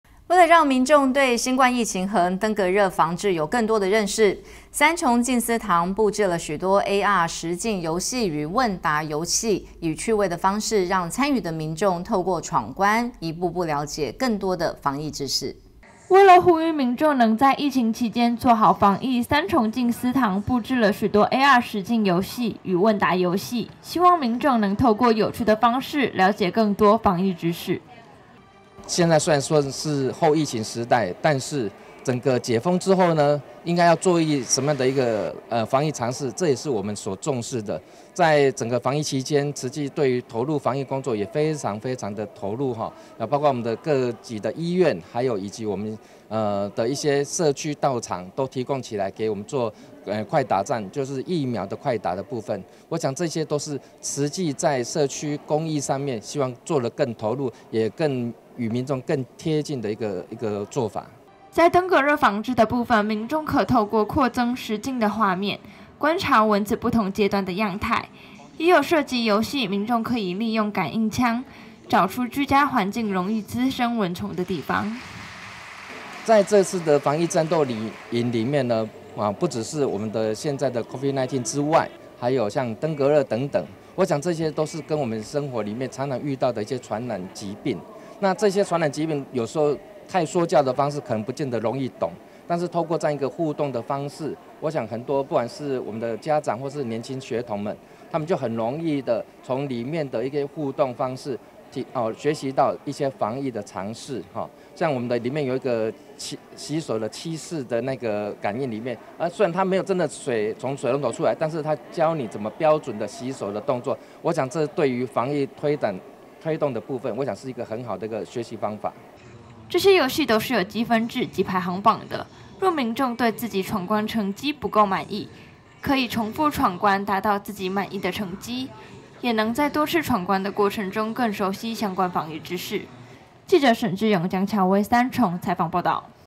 天外天新聞1120303 10靜思堂規劃遊戲 了解防疫知識
為了讓民眾對新冠疫情和登革熱防治有更多認識，三重靜思堂佈置了許多AR實境遊戲與問答遊戲，以趣味的方式，讓參與的民眾透過闖關，一步步了解更多防疫知識。